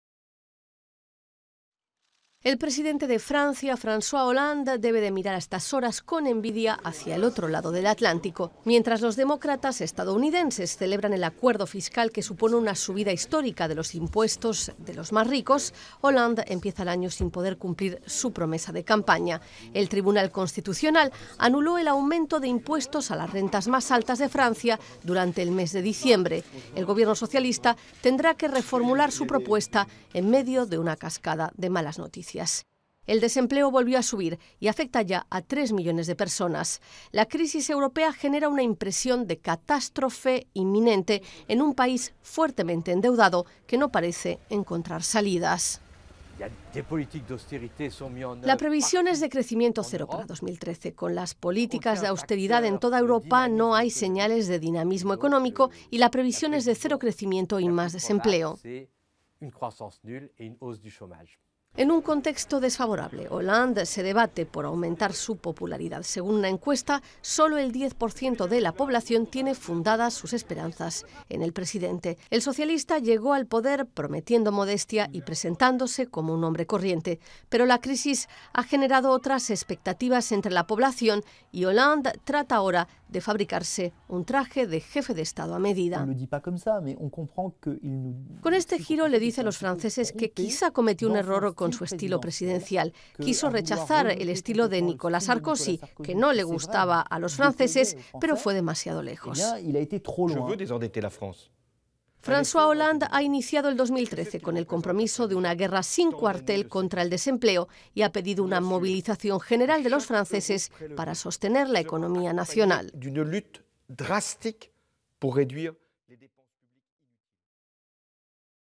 El año más difícil de Hollande
Mientras en Estados Unidos los demócratas lograron subir los impuestos a las rentas más altas, en Francia el presidente Francois Hollande se ve forzado a reformular su reforma fiscal en un año 2013 que se anuncia especialmente difícil.